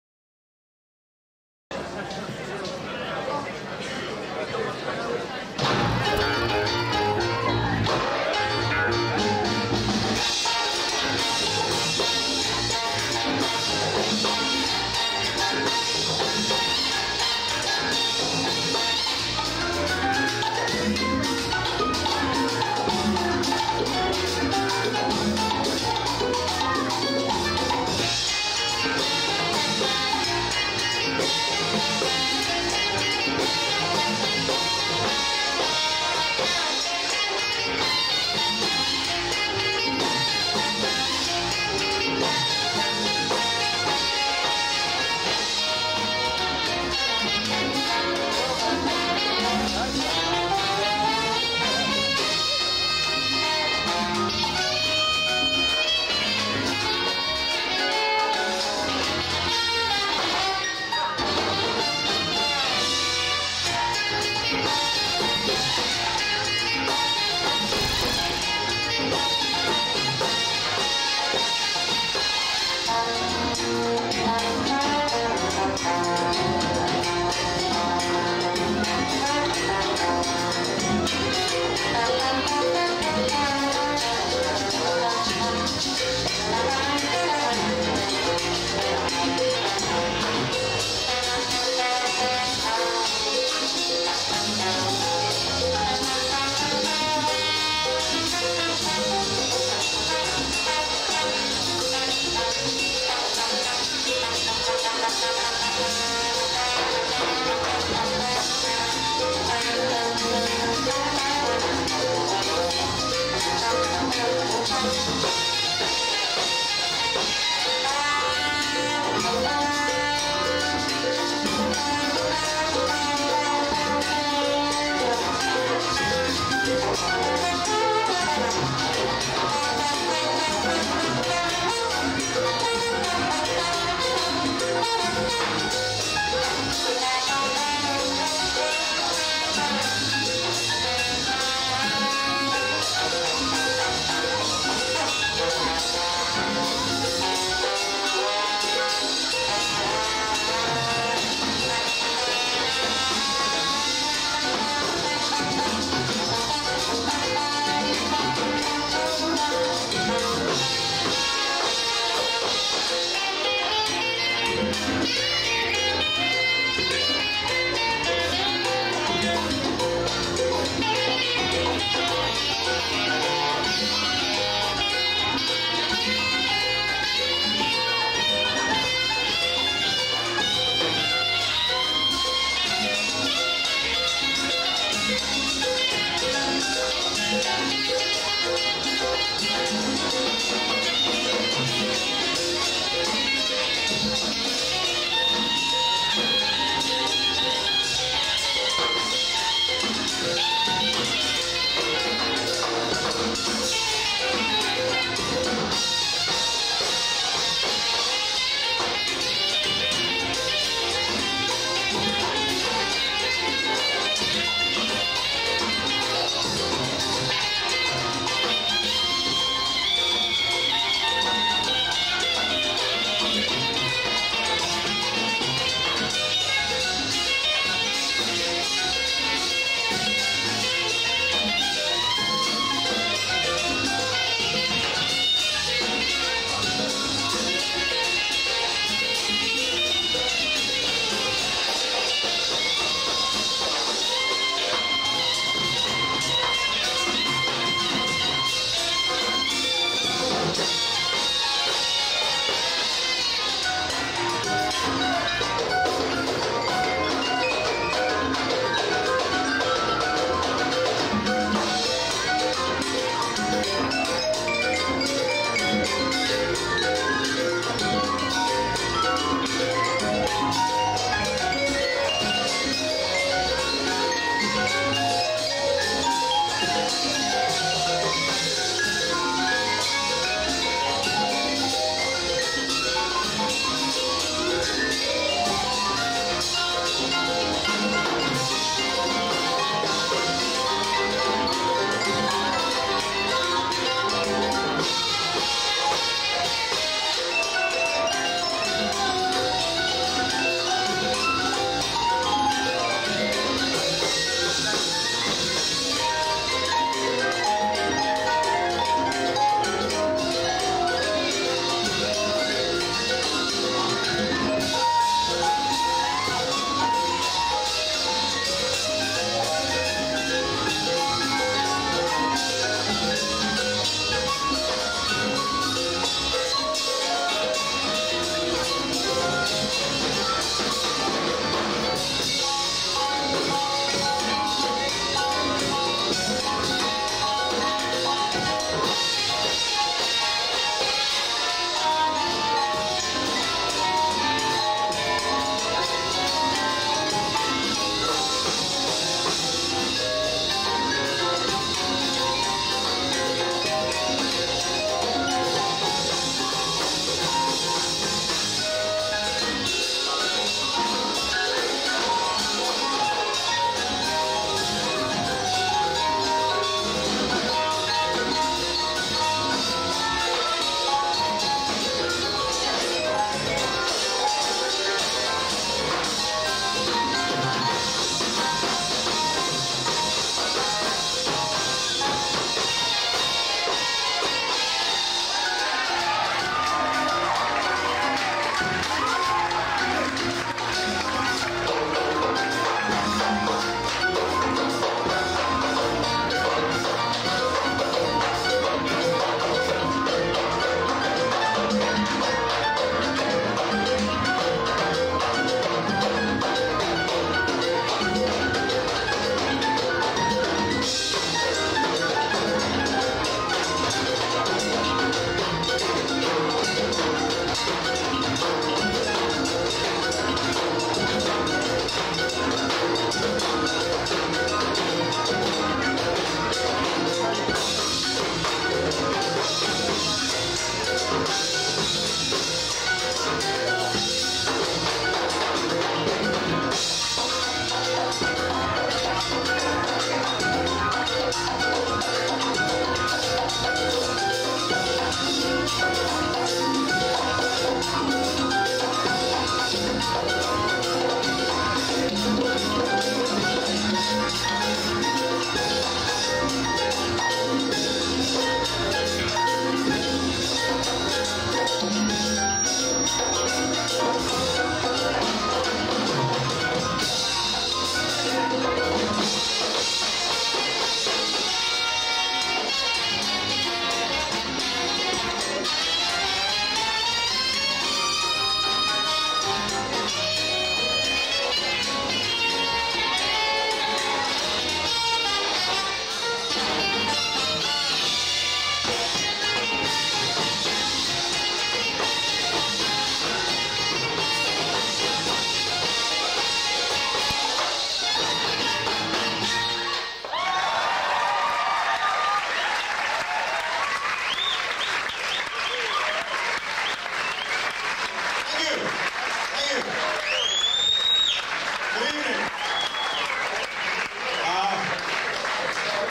APITOS GIG BAND ON THE WALL 1995
Great band that used to play a regular gig at the BOW ( how times have changed)